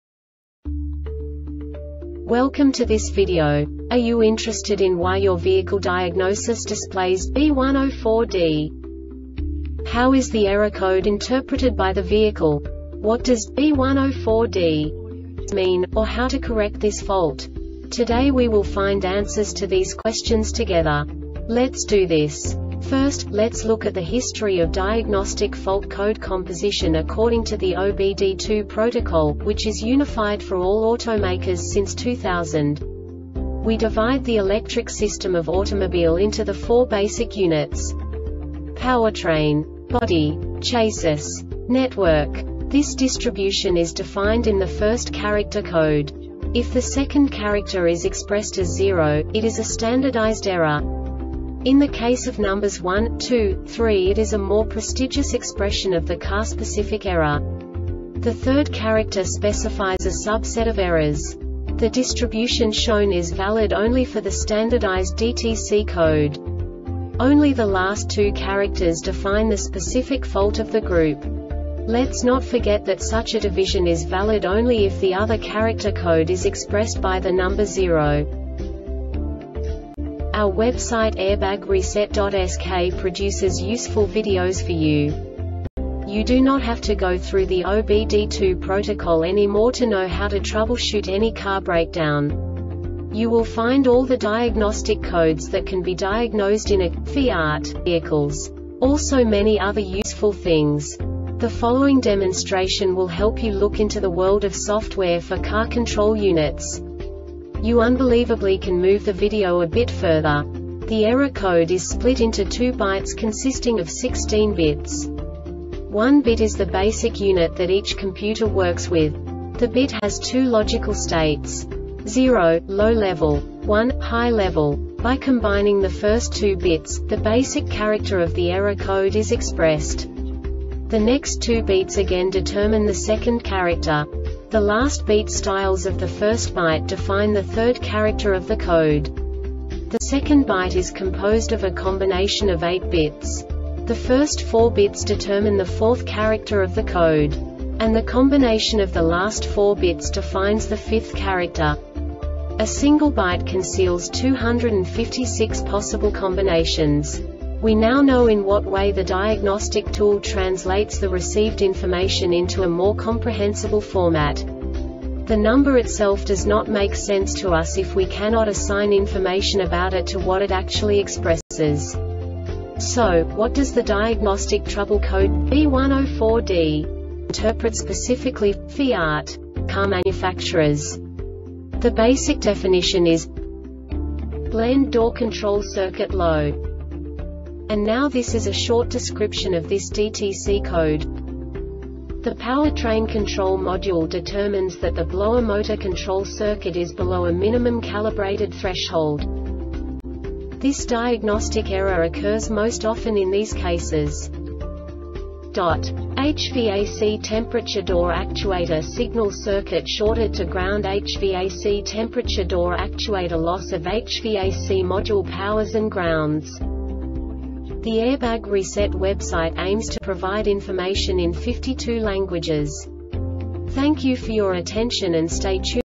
DTC Fiat B104D Short Explanation
Contents:
0:21 Basic DTC analysis according to OBD2 protocol standard.
1:48 Insight into programming
2:58 A diagnostically understandable description of the Diagnostic Trouble Code
3:05 Basic error definition

Make:
FIAT
Code:
B104D
Definition:
Blend Door Control Circuit Low
Description:
The Powertrain Control Module determines that the Blower Motor Control circuit is below a minimum calibrated threshold.
Cause:
Hvac Temperature Door Actuator Signal Circuit Shorted To Ground
Hvac Temperature Door Actuator
Loss Of Hvac Module Powers And Grounds